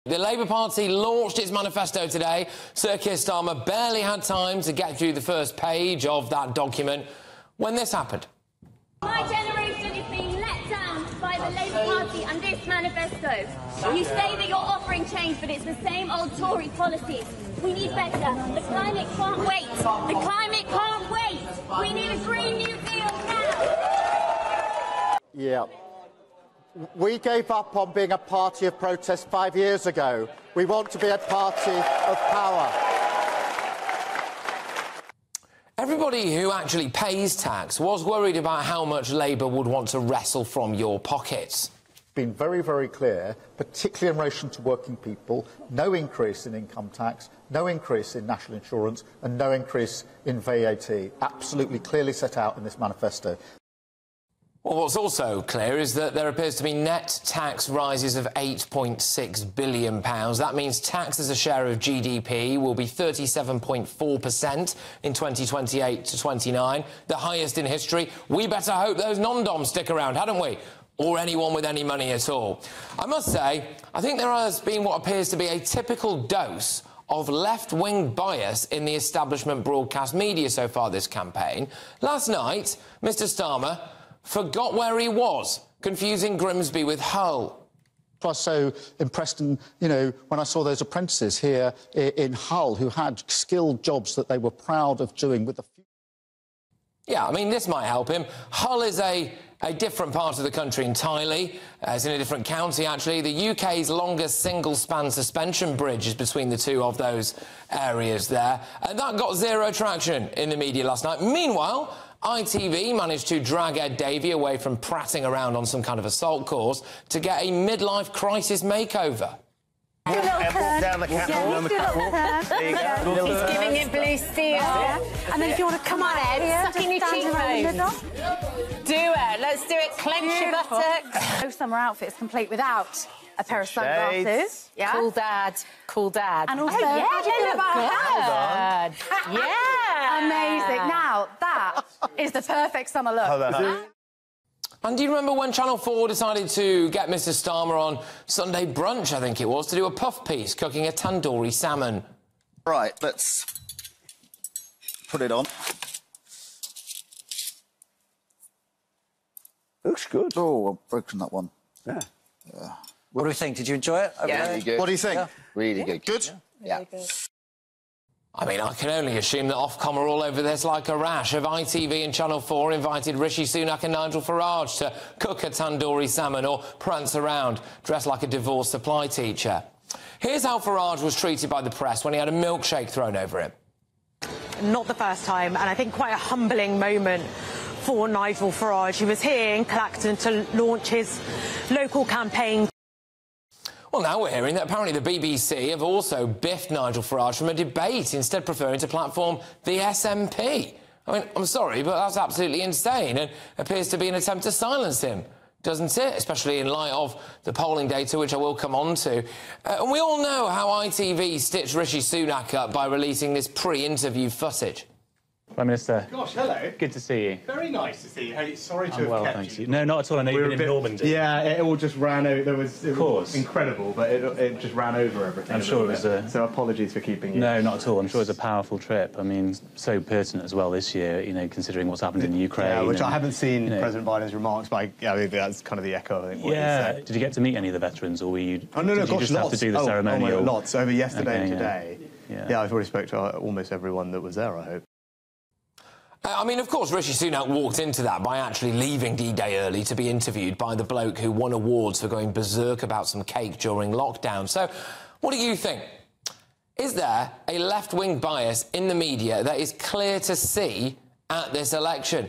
I'm sure Ofcom are ALL OVER this! | Labour's EASY ride proves media bias HATES right-wing voters?!
‘I’m sure Ofcom are all over this! Have ITV and Channel 4 invited Rishi Sunak on to cook salmon and do a catwalk dressed like a divorced supply teacher, like they did with Keir Starmer and Ed Davey?’

Patrick Christys slams establishment media for giving Keir Starmer and Ed Davey easy rides on TV, but seemingly treating Nigel Farage and Rishi Sunak unfairly in their coverage.